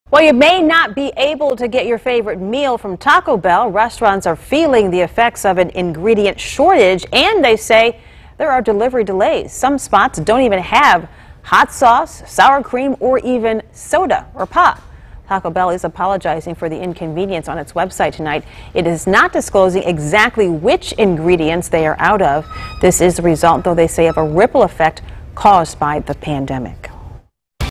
Taco Bell facing a major ingredient shortage
Taco Bell is dealing with an ingredient shortage, leaving some fast food spots without hot sauce, sour cream or soda.